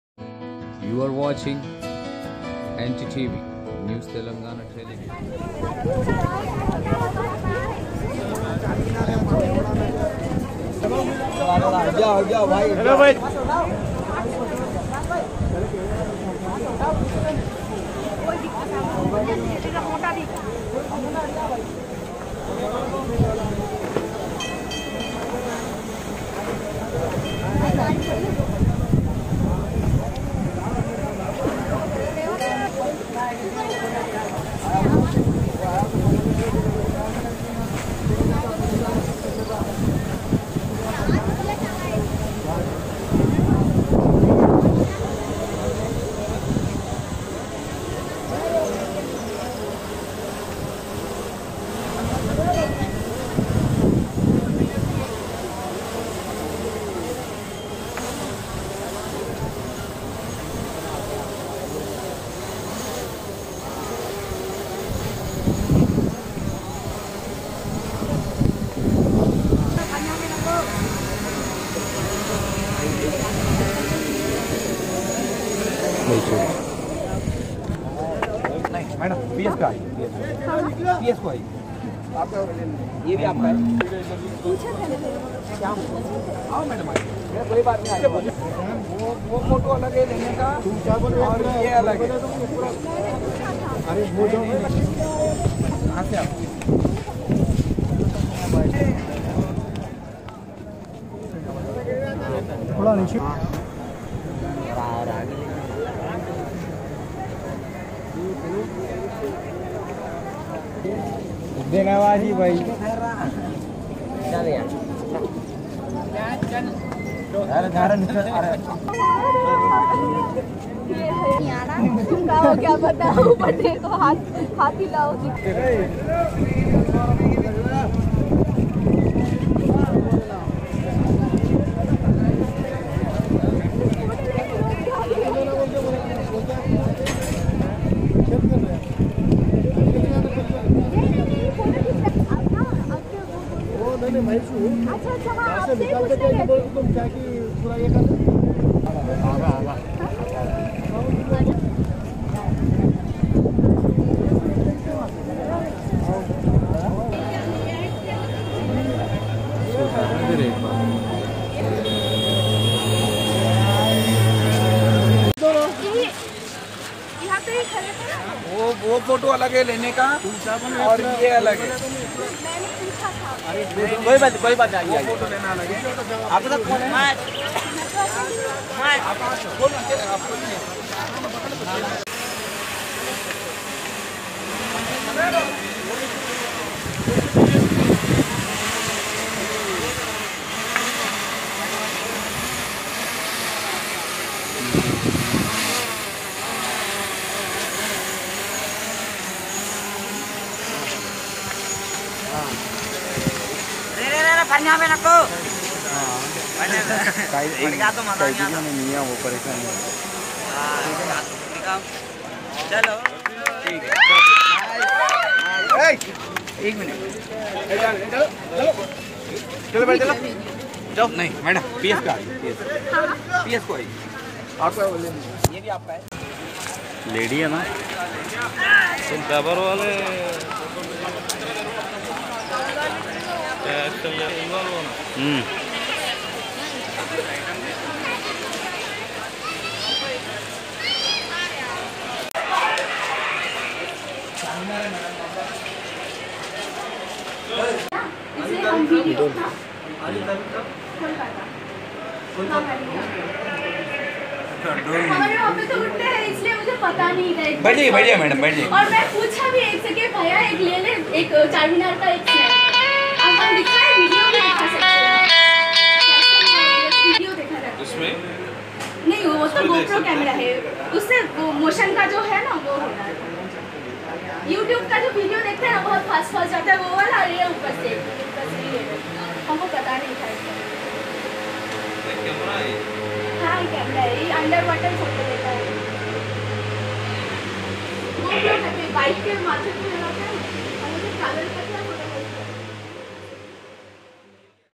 A lady from West Bengal by name Suparna Nath was using Drone camera in the surrounding
A lady from West Bengal by name Suparna Nath was using Drone camera in the surrounding area of Charminar, which is prohibited bye law.

To which the police officials of Charminar reached the spot and  withheld the camera and started investigation.